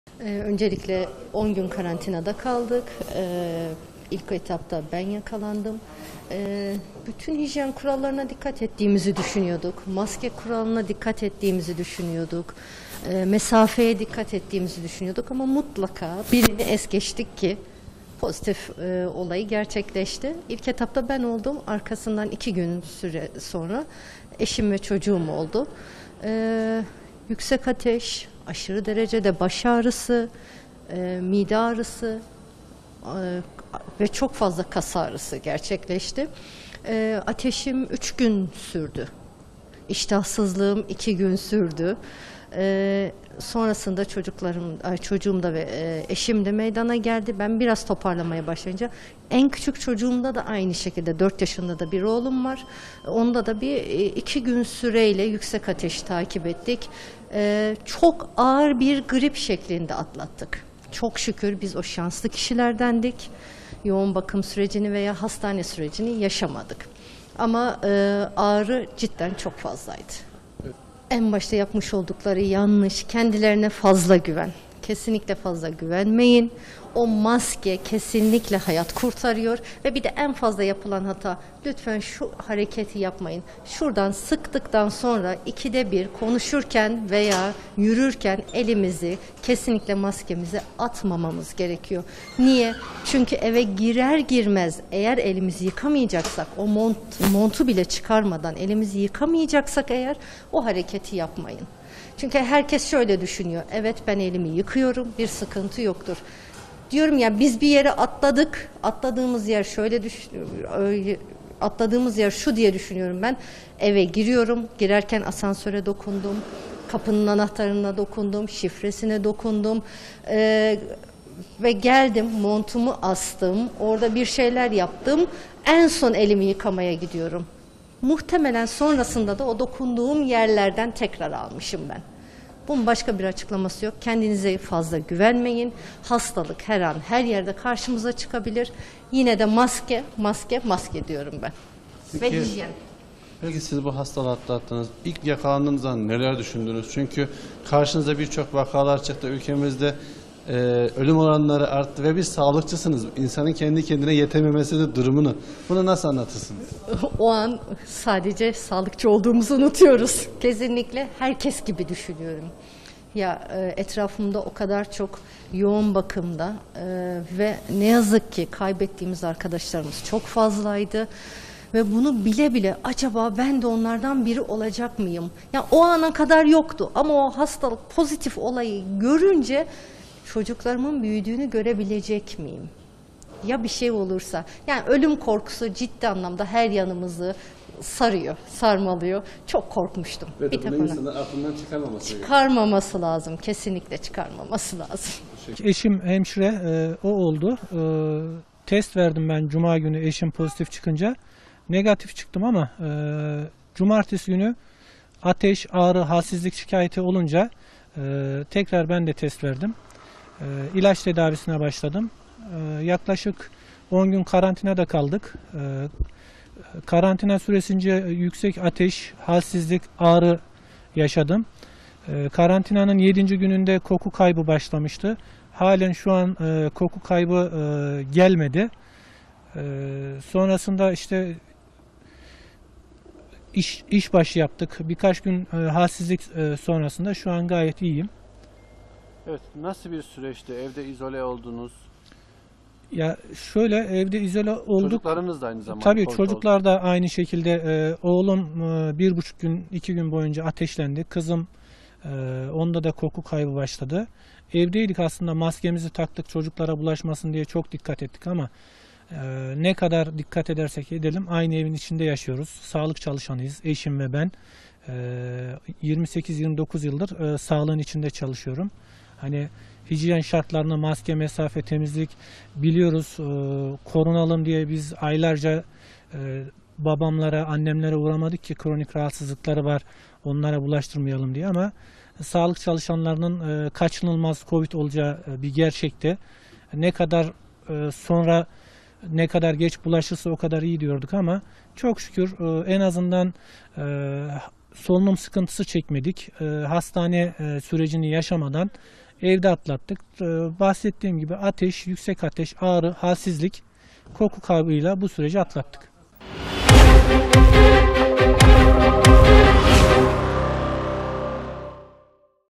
Eşi ve 2 çocuğuyla koronavirüsü yenen hemşire: Maske, insanların hayatını kurtarıyor
KAYSERİ Devlet Hastanesi pandemi servisinde sorumlu hemşire olarak görev yapan Tülay Kök (41), 2 çocuğu ve eşiyle birlikte koronavirüse yakalandı. Tülay Kök, 10 gün süren tedavinin ardından ailesiyle birlikte koronavirüsü yendi. Ancak aynı hastalıktan dedesi İbrahim Tosun'u (83) kaybeden Kök, "İnsanlar kendilerine 'bana virüs bulaşmaz' diye çok fazla güveniyorlar. Bu çok yanlış. Her yerde takılması söylenen maske, insanların hayatını kurtarıyor" dedi.

Kayseri Devlet Hastanesi pandemi servisinde sorumlu hemşire olarak görev yapan 2 çocuk annesi Tülay Kök, 27 Kasım'da yüksek ateş, öksürük ve kas ağrıları nedeniyle koronavirüs testi yaptırdı. Aynı gün testi pozitif çıkan Kök, evde kendini karantinaya aldı. İki gün sonra da Tülay Kök'ün İl Sağlık Müdürlüğü'nde teknik uzman olarak çalışan eşi Reşat Kök (42) daha sonra da çocukları Kaan Mert (4) ve Elif Su (8) koronavirüse yakalandı. Evde kendilerini karantina altına alan aileden önce çocuklar sonra da Tülay-Reşat Kök çifti hastalığını yendi. 10 gün süren tedavinin ardından ailesiyle birlikte koronavirüsü yenen Tülay Kök, 7 Aralık'ta göreve başladı.

'YOĞUN BAKIM VE HASTANE SÜRECİ YAŞAMADIK'

Tülay Kök, hastalığa yakalanmadan önce dedesi İbrahim Tosun'u koronavirüs nedeniyle kaybettiklerini söyleyerek, "Bütün hijyen kurallarına ailece uyduğumuzu düşünüyorduk. Maalesef bir yerde hata yaptık. Önce ben koronavirüse yakalandım sonra da eşim. Bizden sonra da çocuklarımıza bulaştı. Çok ağır grip şeklinde atlattık. Yoğun bakım ve hastane süreci yaşamadık. Ağrılarımız çok fazlaydı. İnsanlar kendilerine 'bana virüs bulaşmaz' diye çok fazla güveniyorlar. Bu çok yanlış. Her yerde takılması söylenen maske, insanların hayatını kurtarıyor. Virüsten korunmak için elinizi yıkamak tek başına yetmiyor. Ben dokunduğum bir yerde virüsü kapmış olduğuma inanıyorum.  Bu hastalık her yerde karşınıza çıkabilir" dedi.

'İNSANLAR TEDBİRLERE UYSUNLAR'

Tülay Kök'ün eşi Reşat Kök ise, koronavirüse önce eşinin yakalandığını söyleyerek, "Eşimden iki gün sonra da bende ateş, baş ağrısı oluştu. Yaptırdığım testimin sonucu pozitif olunca karantinada kaldım. Evdeki karantinamın 7'nci gününde de koku kaybı yaşadım. Hala koku kaybı yaşıyorum. Eşim ve bende koronavirüs çıkınca çocuklarımız da koronavirüse yakalandı. İnsanlar açıklanan tedbir kurallarına uysunlar. Maske takmayarak kimse bir başkasının hayatını riske etmesin" diye konuştu.